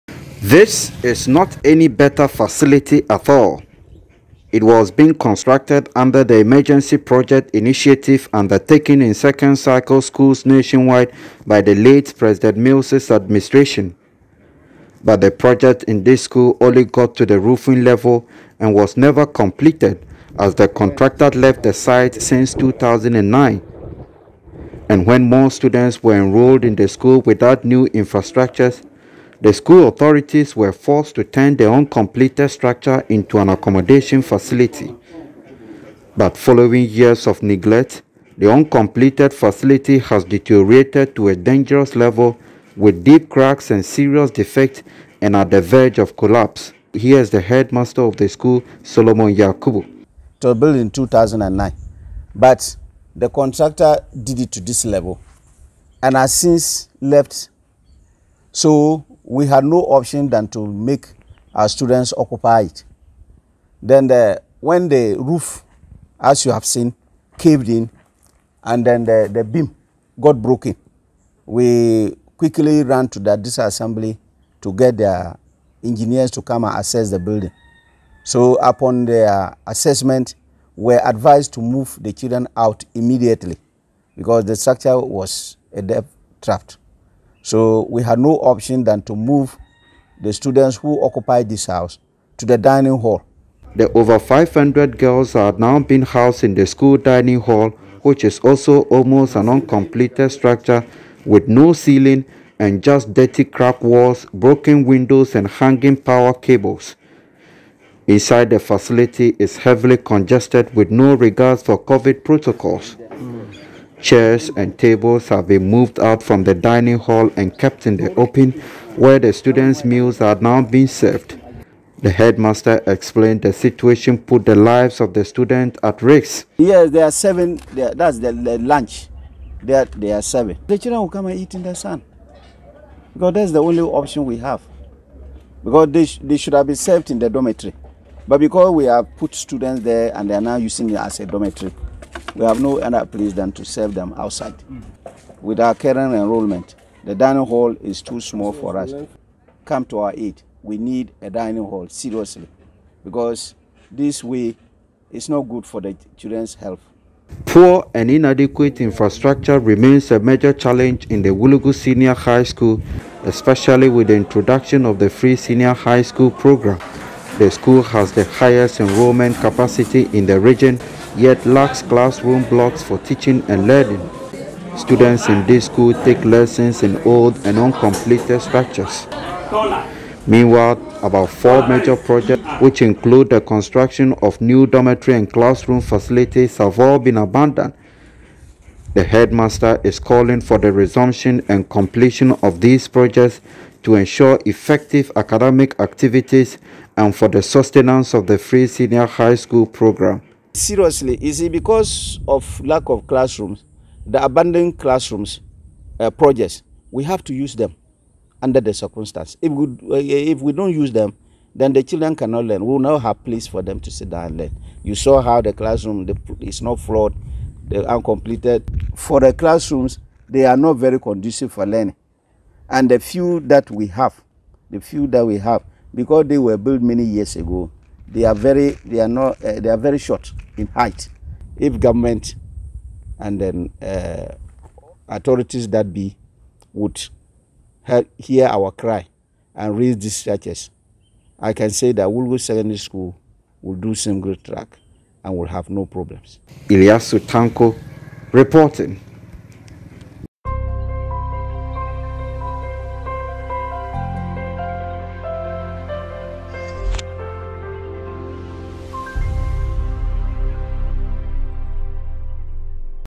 Wulugu SHS call on gov't to fix poor and inadequate school building blocks
The headmaster of Wulugu Senior High School has called on the government to fix the inadequate and deteriorated infrastructures in the school.